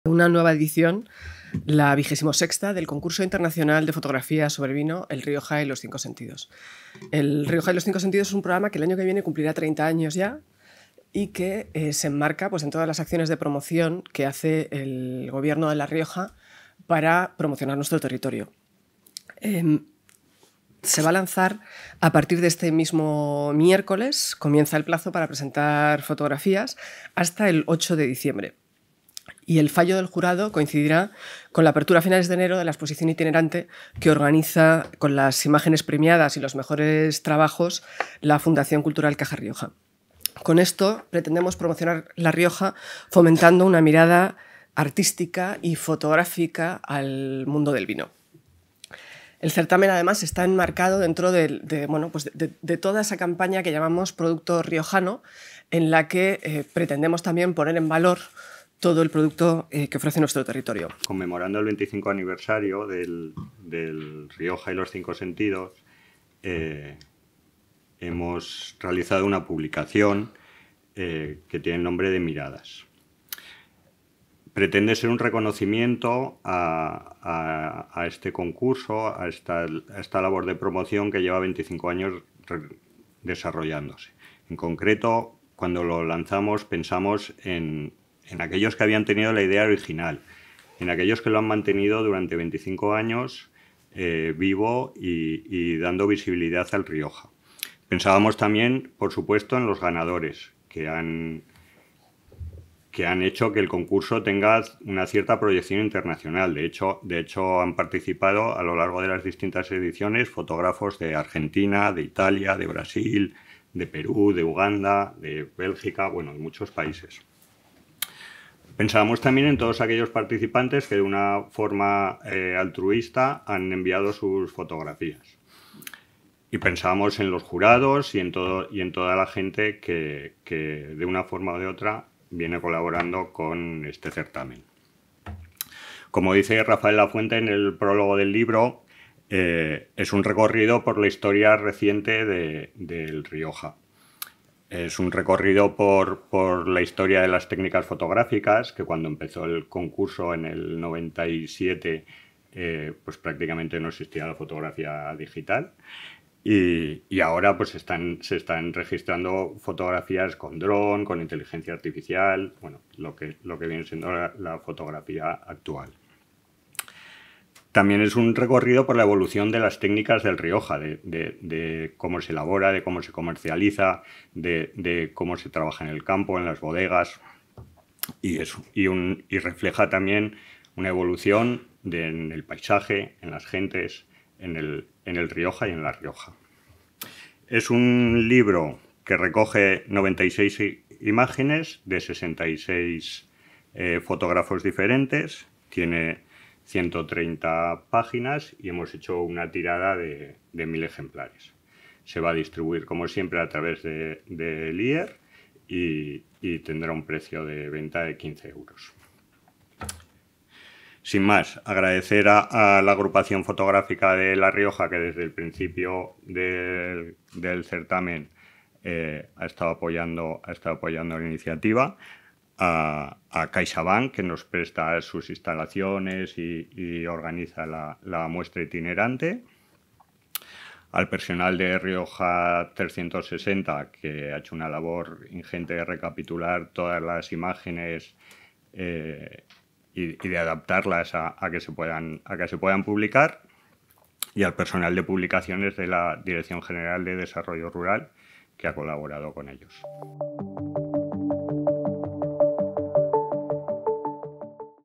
XXVI Concurso Internacional de Fotografía sobre Vino ‘El Rioja y los 5 Sentidos’
El director general de Desarrollo Rural, David Martín, junto con la directora general de Turismo y Promoción Territorial, Virginia Borges, presentan el XXVI Concurso Internacional de Fotografía sobre Vino ‘El Rioja y los 5 Sentidos’.